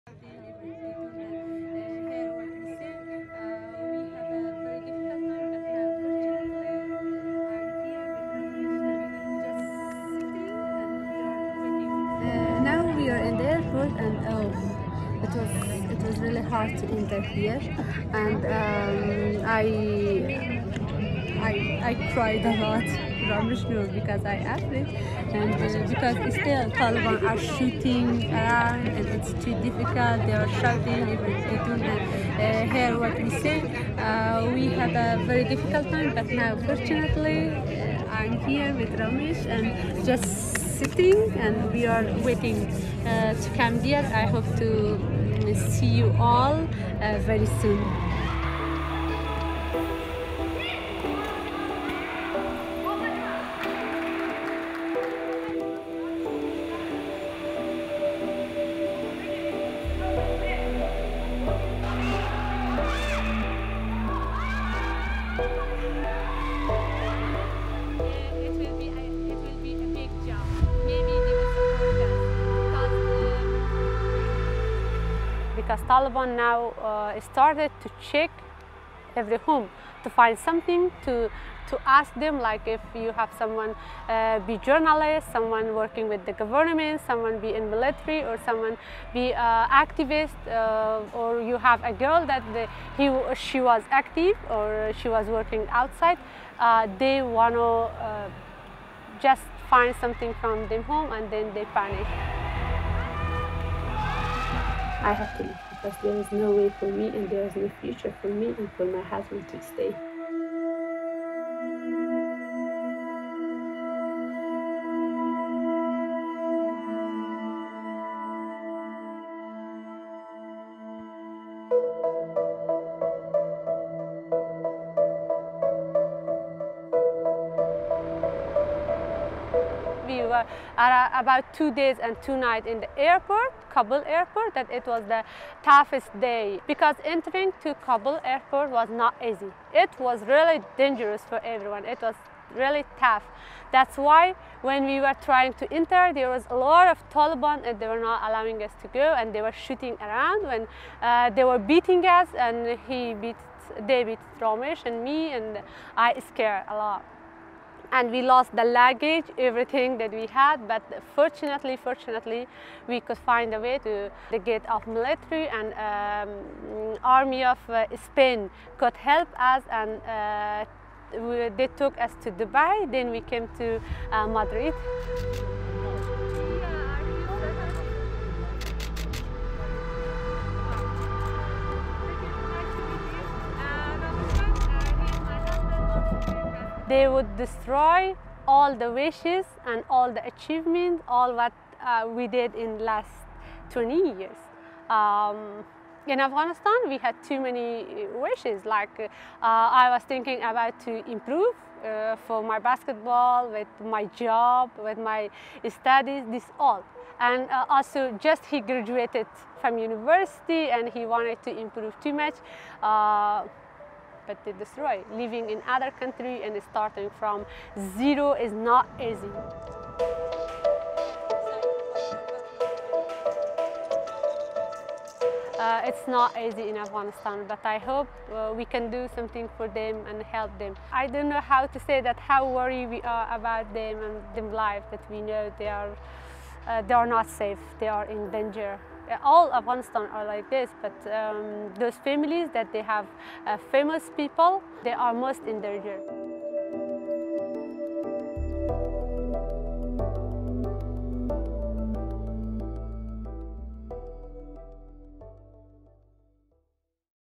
Fleeing Kabul: Female Afghan wheelchair basketball captain’s journey to Spain
Afraid of being targeted by the Taliban, Nilofar Bayat, captain of Afghanistan’s female wheelchair basketball team, fled to Spain with her husband, Ramesh, on August 20, 2021. The Spanish government promised to try to evacuate the couple after a journalist shared their story on Twitter. The 28-year-old has also been selected to join the wheelchair basketball team in the northern Spanish city of Bilbao.